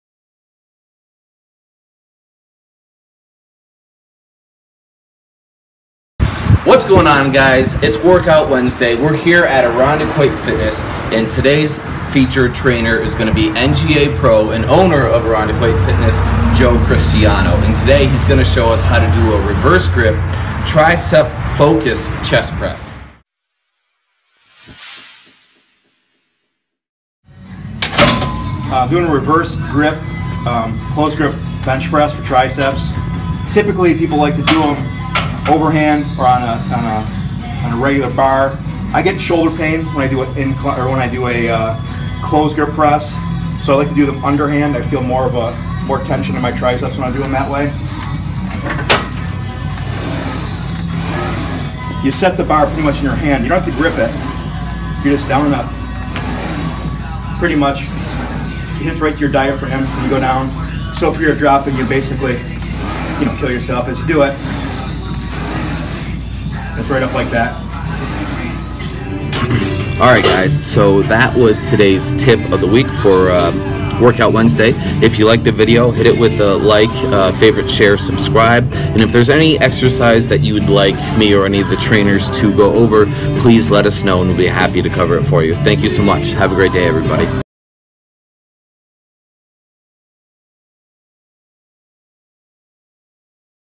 REVERSE GRIP TRICEP PRESS - NGA PRO BODYBUILDER JOE CHRISTIANO - WORKOUT WEDNESDAY
How to, and tip on a variation of the close grip tricep press
Paramountperformfit.com
Paramount performance FACEBOOK
Paramount performance INSTAGRAM
Hockeyrocker76 snapchat
JBRDapparel.com